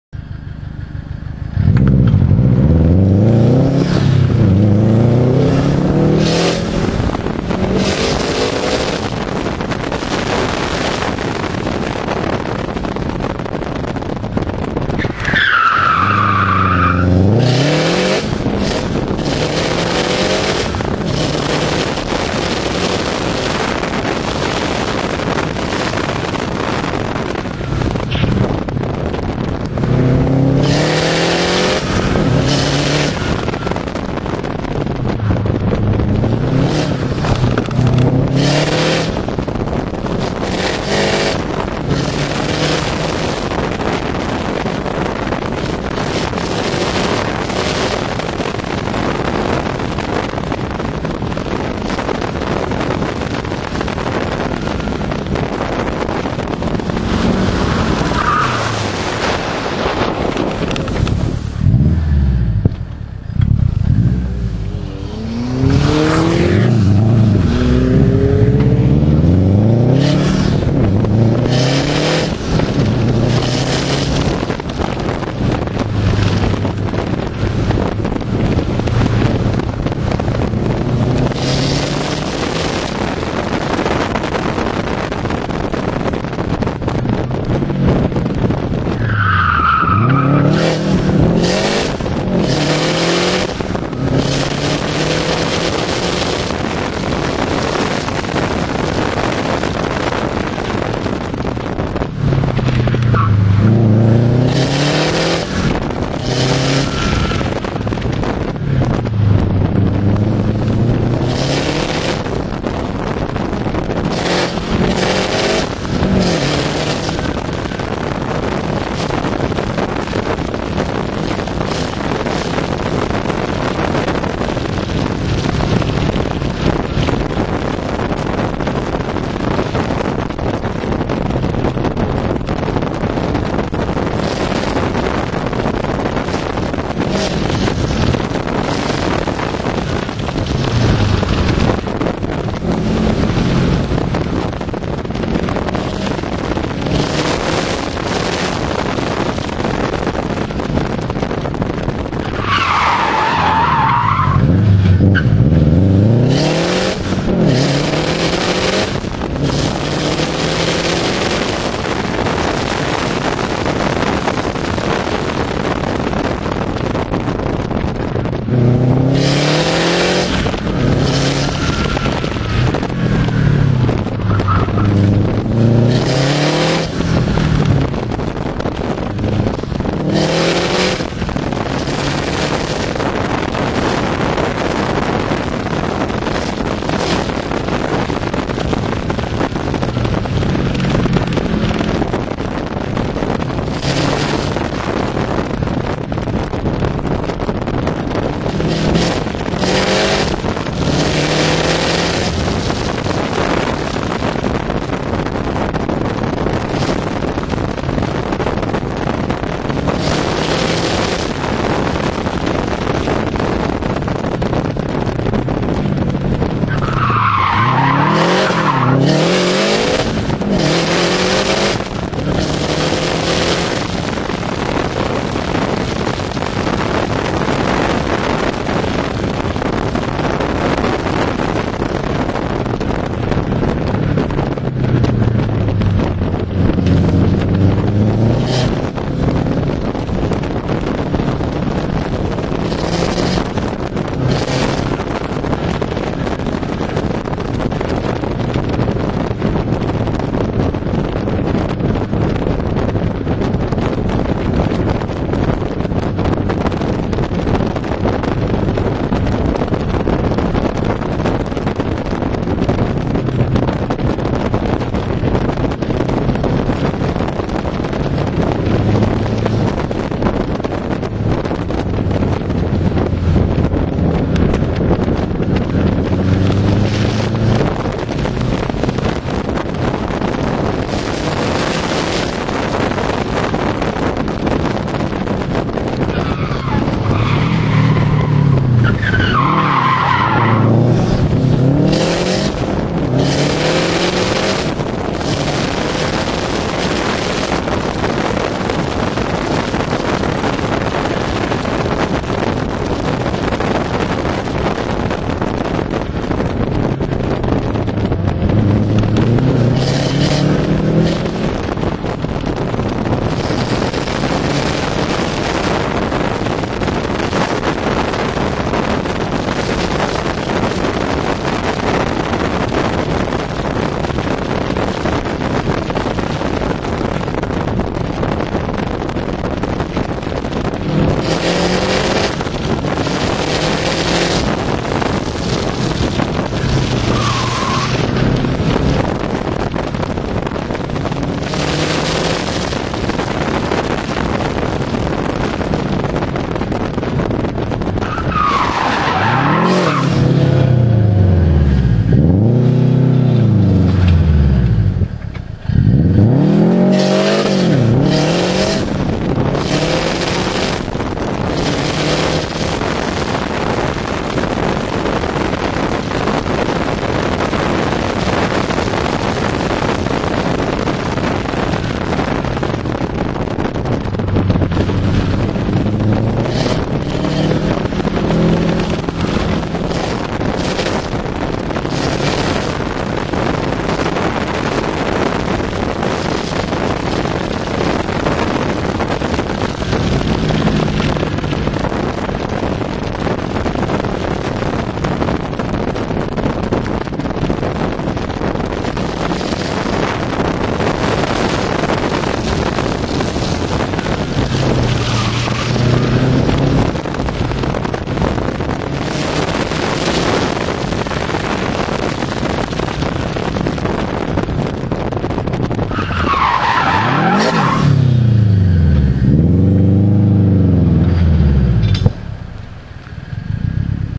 Bělá pod Bezdězem: David
AWDOC.cz člen týmu David v akci na uzavřeném okruhu v Bělé pod Bezdězem ... AWDOC.cz team member David in action at the closed course Bělá pod Bezdězem - v / in 2006 Subaru STi